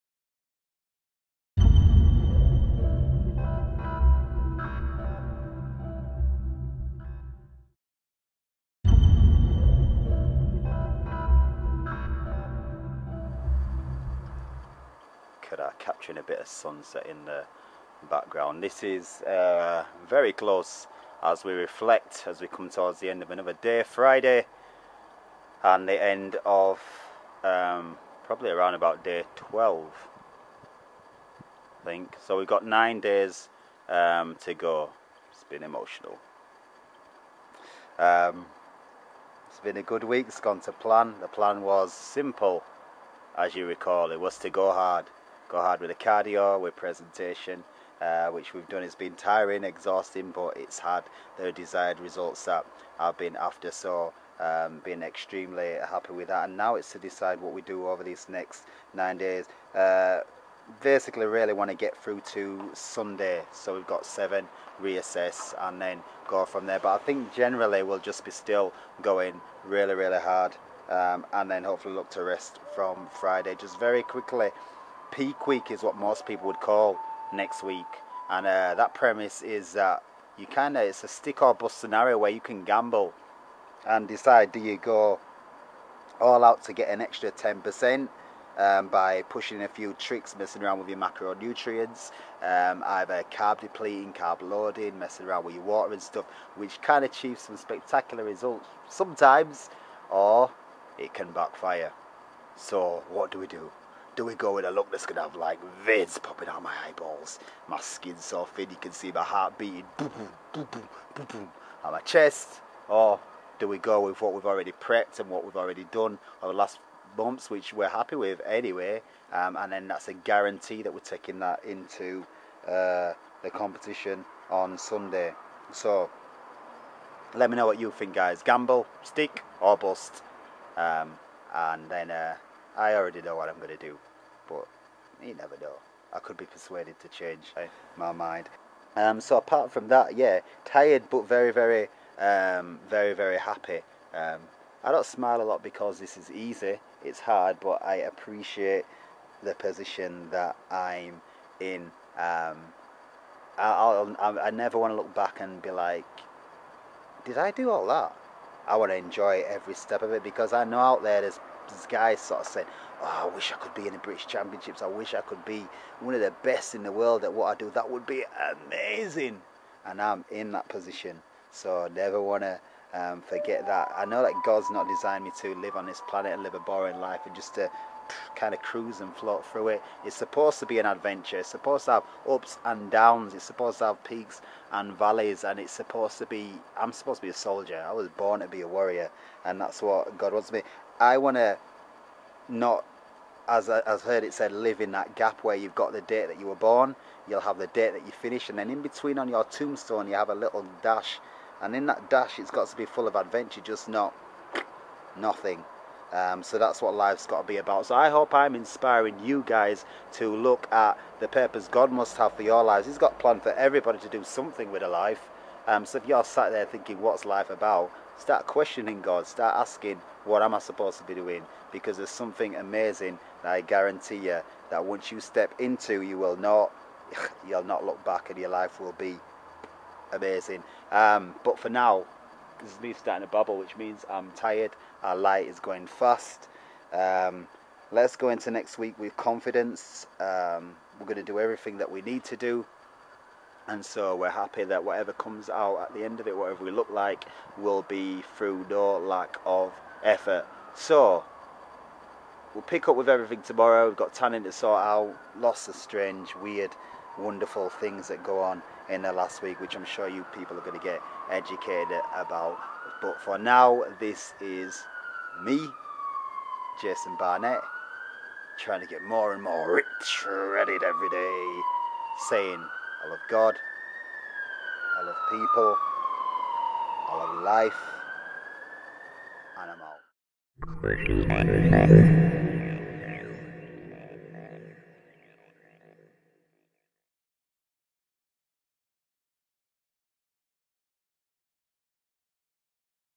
JB's Bodybuilding Contest countdown 9 days out.
World Natural bodybuilding Champion Jason barnett Counts down daily to the British Natural bodybuilding Finals as part of the Deeper Underground series.